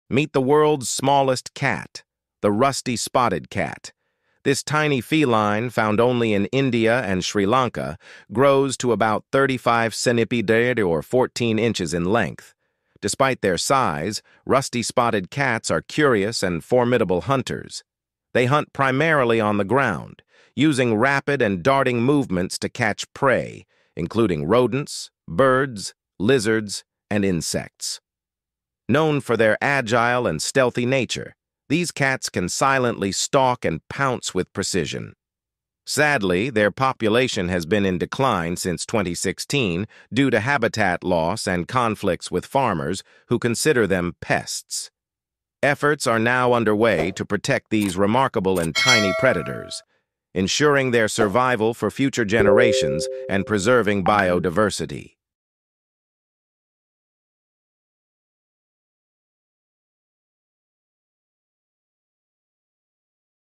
Rusty spotted Cat | The World's Smallest Cat
Meet the world's smallest cat: the rusty-spotted cat. This tiny feline, found only in India and Sri Lanka, grows to about 35 cm or 14 inches in length. Despite their size, rusty-spotted cats are curious and formidable hunters. They hunt primarily on the ground, using rapid and darting movements to catch prey, including rodents, birds, lizards, and insects. Known for their agile and stealthy nature, these cats can silently stalk and pounce with precision. Sadly, their population has been in decline since 2016 due to habitat loss and conflicts with farmers, who consider them pests. Efforts are now underway to protect these remarkable and tiny predators, ensuring their survival for future generations and preserving biodiversity.